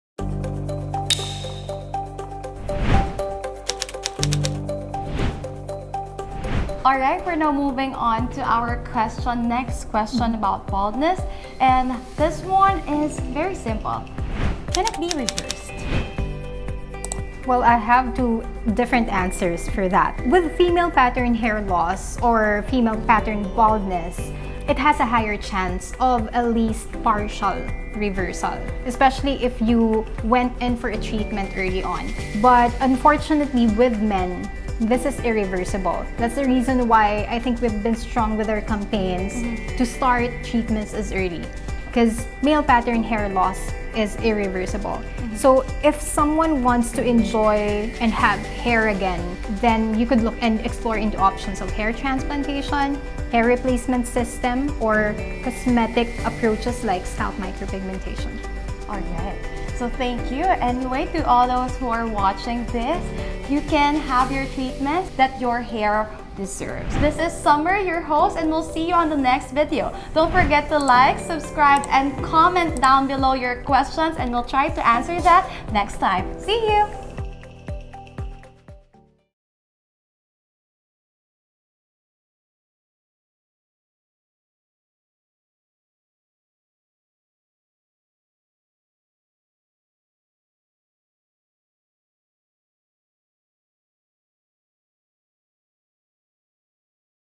Trichologist Answers Google Questions | Episode 25: Can baldness be reversed?
Can baldness be reversed?

Today, we're diving headfirst into the age-old question that has intrigued humanity for centuries: "Can Baldness Be Reversed?"

Don't miss out on the enlightening answers to the most Googled questions about hair and scalp concerns!